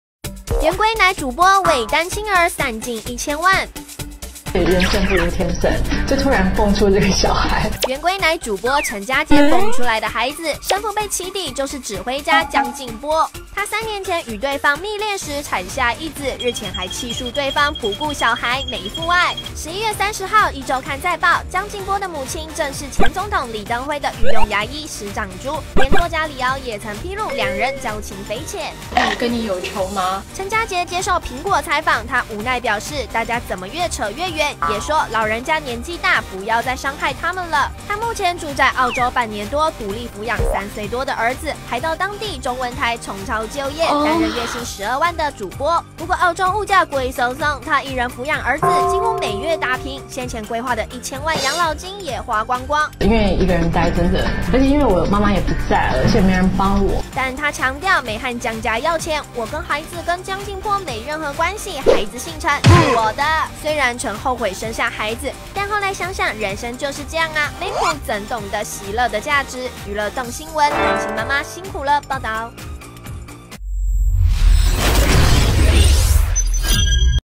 圓規奶主播 陳珈婕 曾落魄靠買地瓜果腹 養兒花光千萬不後悔
圓規奶主播情路坎坷落魄買地瓜果腹 圓規奶主播曾落魄靠買地瓜果腹養兒花光千萬不後悔 圓規奶主播私生子風波牽扯前總統李登輝？42歲身材姣好被封為「圓規奶」的前主播陳珈婕，3年前未婚生子，而孩子的爸則是知名指揮家江靖波，結果江靖波卻認為兩人沒有交往，只是因為喝酒誤事發生的「一夜情」，讓陳珈婕痛批說謊。

而根據報導，陳珈婕情路坎坷，演藝事業也不順遂，當年她卸下主播光環後轉戰當藝人，結果發生整形糾紛又提前結束經紀約，讓她長達10個月沒收入，甚至落魄到買地瓜果腹。而如今為了兒子也花光1000萬的養老金，雖然江靖波每月都會匯1萬5000元到小孩帳戶，但陳珈婕不領情，認為「那是他以後爭取小孩監護權的做法，我不予置評」。

另外為了照顧小孩，得遠赴異鄉打拼，對此會不會後悔生下小孩，陳珈婕也坦承曾經後悔過，但後來想想「人生若沒苦，怎麼懂珍惜樂的價值？」讓她選擇好好面對挑戰。